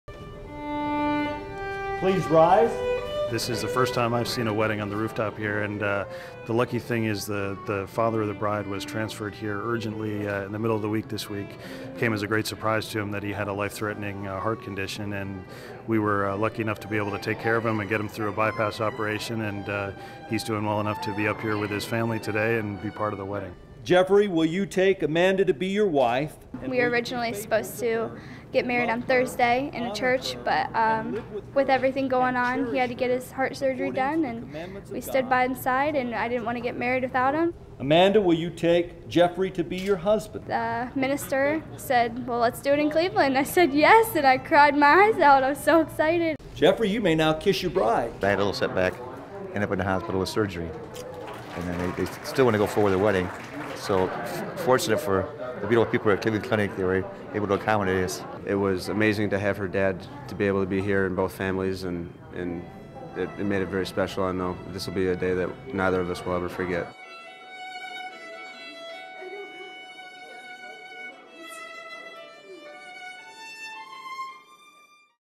Valentine's Day Comes Early to Cleveland Clinic
Nurses in Cleveland Clinic's Miller Family Heart & Vascular Institute helped make a dream come true. Amanda Wick, a Bowling Green State University student, planned to marry U.S. Army Staff Sgt. Geoffrey Pryka before his tour of duty in Afghanistan.  The wedding was postponed due to her father, Timothy Finneran, being rushed to Cleveland Clinic for heart surgery.  After a quadruple bypass, Amanda's father was well enough to go down the isle with her.